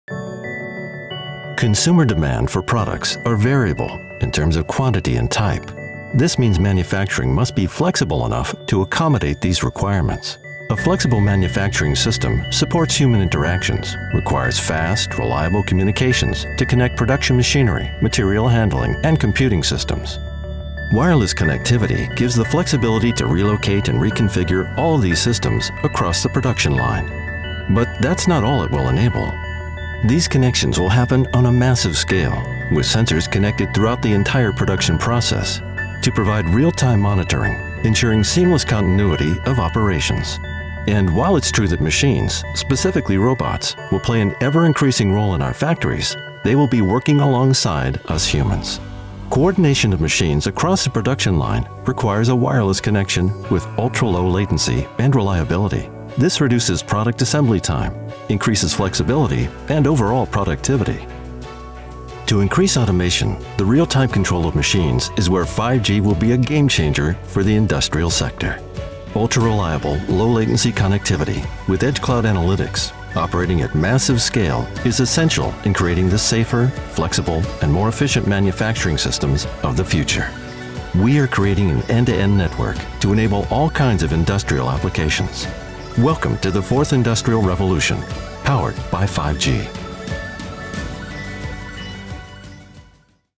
5G will power the 4th industrial revolution
Nokia is creating an end-to-end network to enable all kinds of industrial applications. This video explains why 5G will be a game changer for the industrial sector.